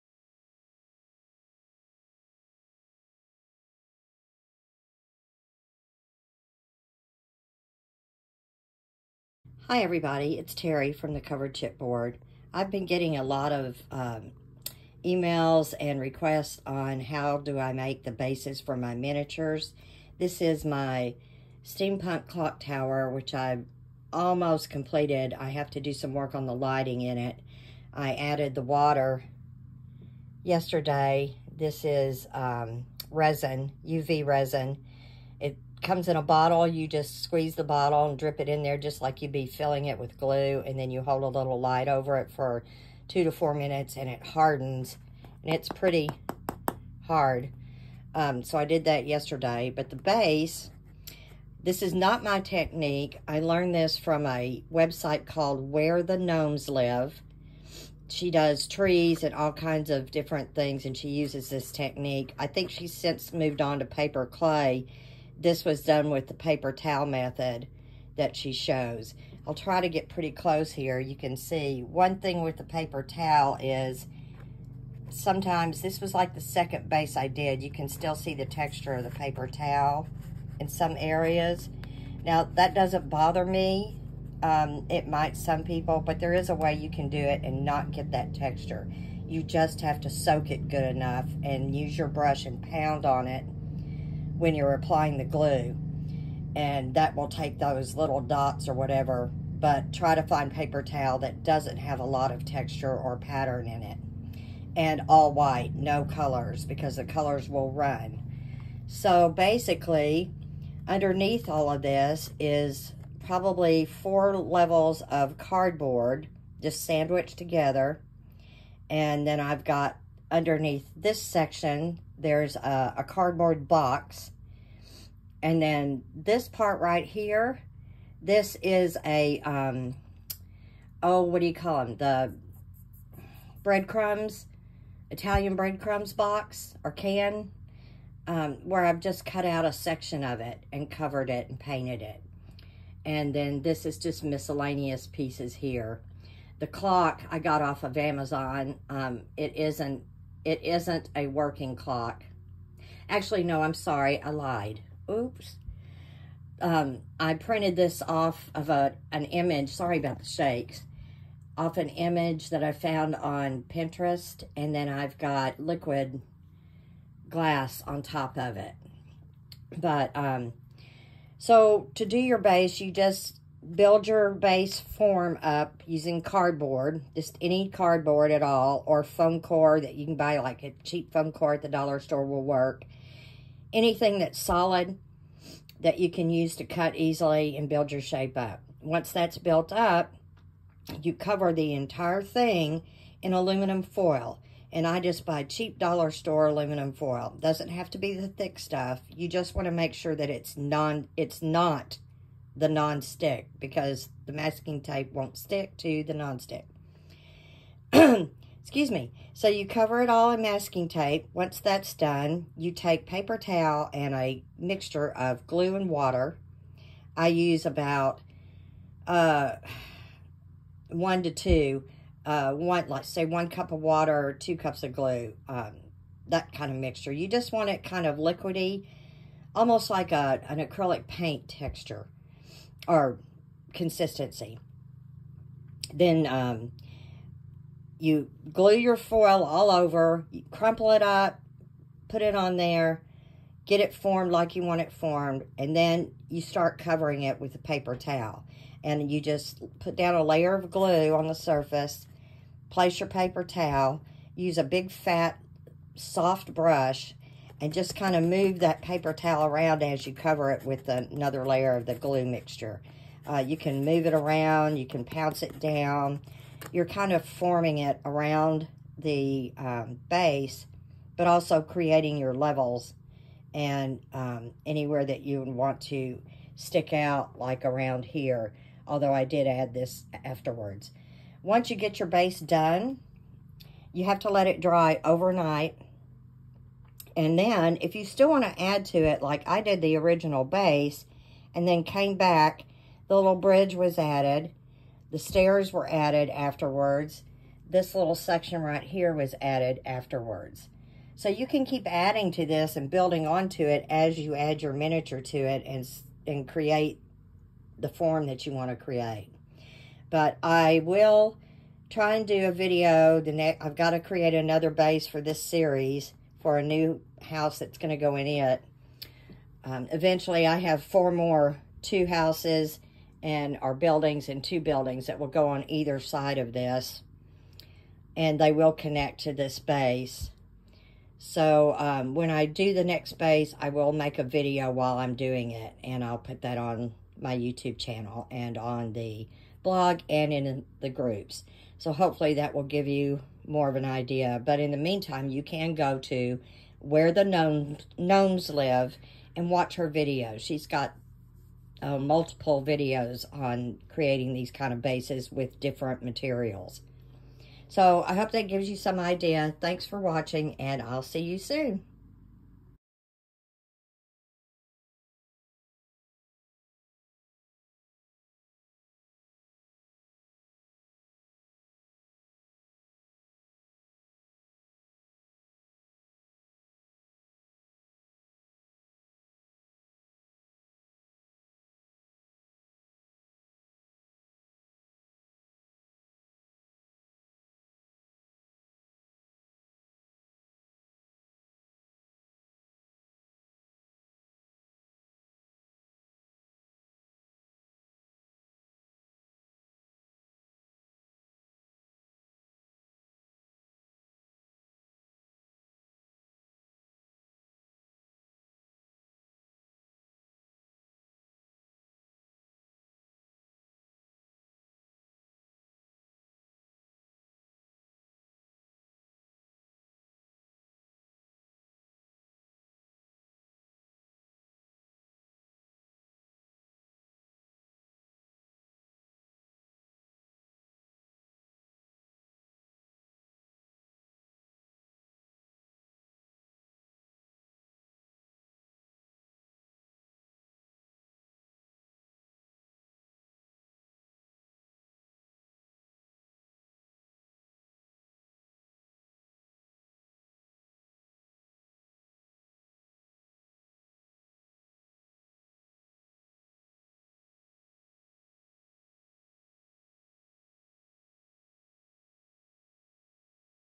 Clock Tower Base
This is a short video about how I create bases for my miniatures. I learned the technique from Where The Gnomes Live so be sure and visit her site and watch her videos! I'll be adding a new video as I create a new base soon!

Teri Hanson
The Covered Chipboard